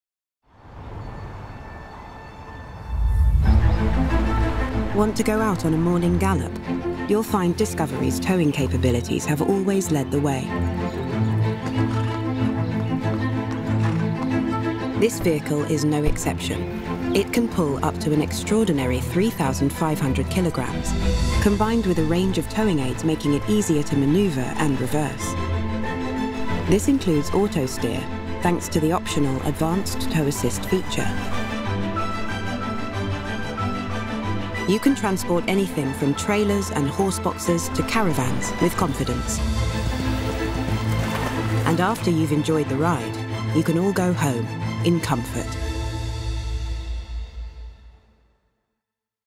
Discovery | Towing
Land Rover Discovery’s towing capabilities have always led the way. With a best-in-class 3,500kg towing capacity and a range of towing aids like Advanced Tow Assist technology to take the stress out of towing manoeuvres, you can transport anything from trailers and horse boxes to caravans with confidence.